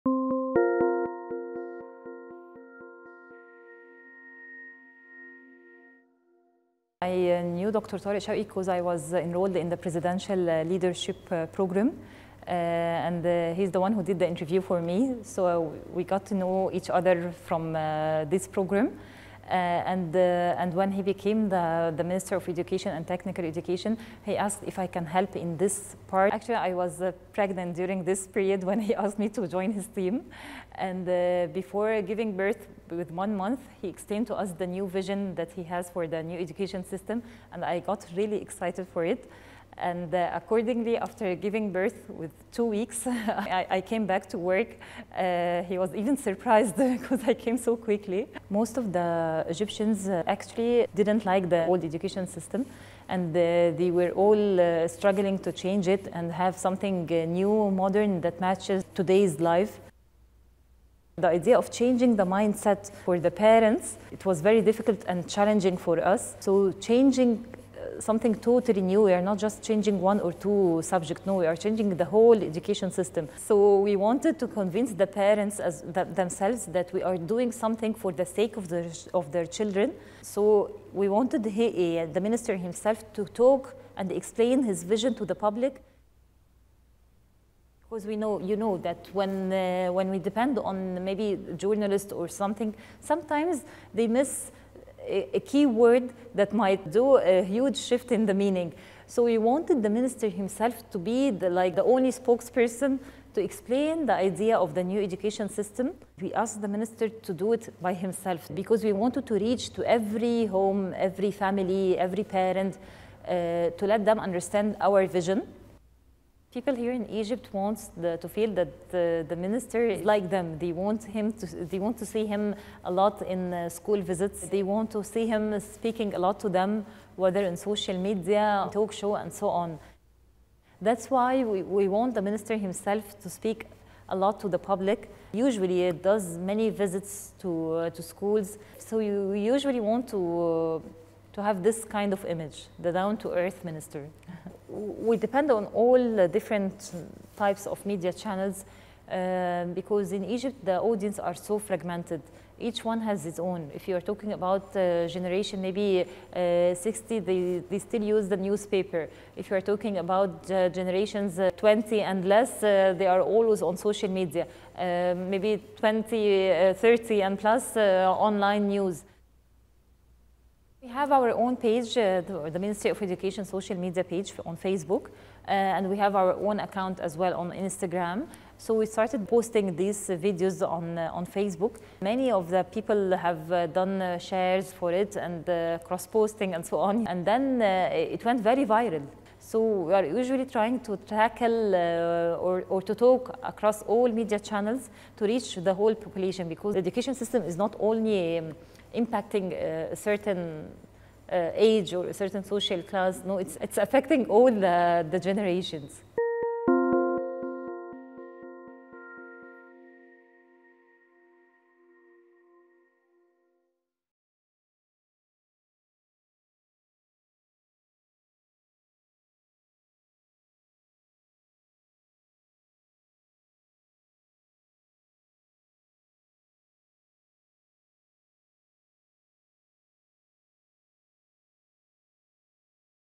Yousra Allam: Media Strategy for Education Reform
Dr. Yousra Allam, Marketing and Communication Advisor to H.E. the
Egyptian Minister of Education,
  Dr. Tarek Shawki,  explains the media strategy to communicate  the Education 2.0 reform to the Egyptian public.

Interview by Linda Herrera, recorded on March 11, 2020 at the Ministry of Education and Technical Education, Cairo.

This video is part of a series on the Oral History of Education 2.0 of the Education 2.0 Research and Documentation Project.